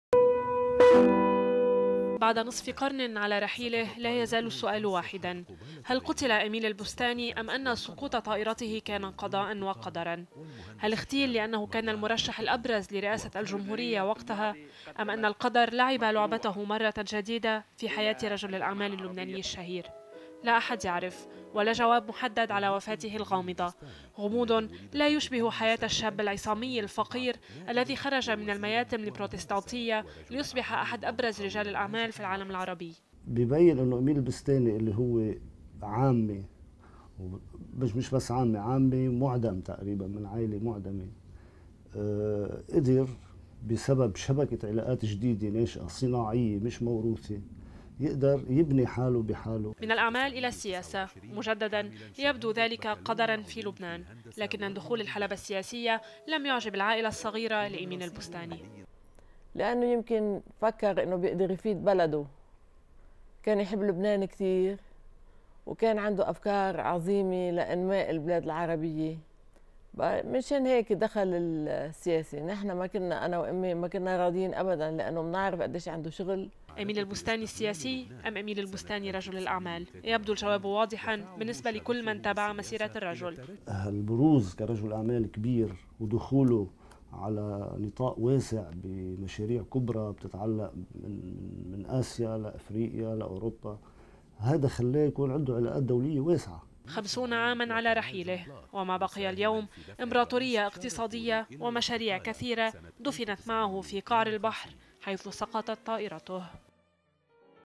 LBCINews-50 عاماً على غياب السياسي إميل البستاني
بعد نصف قرن على رحيله لا يزال السؤال يتردد : هل قتل إميل البستاني أم أن سقوط طائرته كان قضاء وقدراً؟ هل اغتيل لأنه كان المرشح